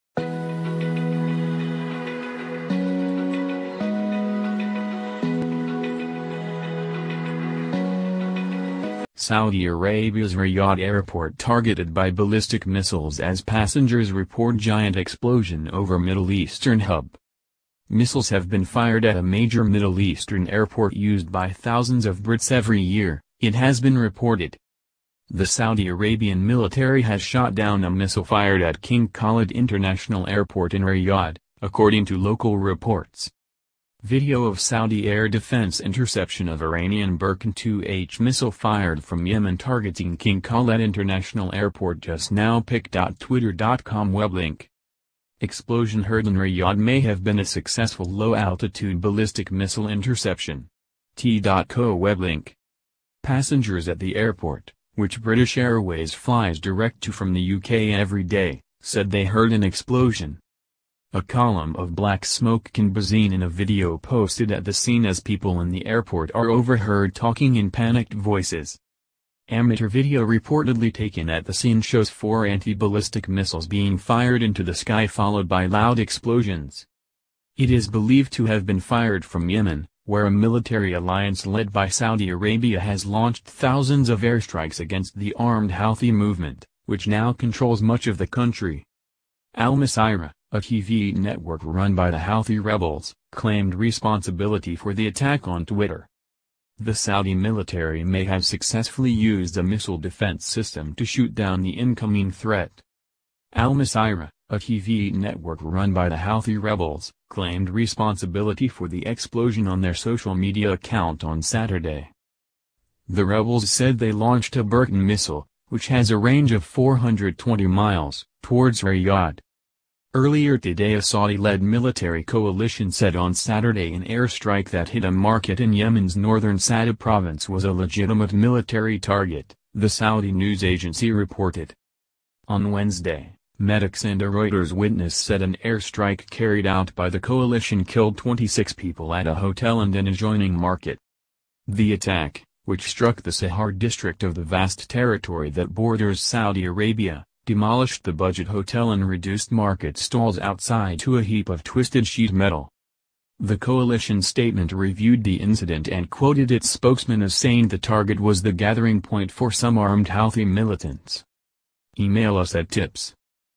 Saudi Arabia’s Riyadh Airport targeted by ballistic missiles as passengers report giant
Saudi Arabia’s Riyadh Airport targeted by ballistic missiles as passengers report giant explosion over Middle Eastern hub.
MISSILES have been fired at a major Middle Eastern airport used by thousands of Brits every year, it has been reported.

The Saudi Arabian military has shot down a missile fired at King Khalid International Airport in Riyadh, according to local reports.

Video of Saudi Air Defense interception of Iranian Burkan 2H missile fired from Yemen targeting King Khaled International Airport just now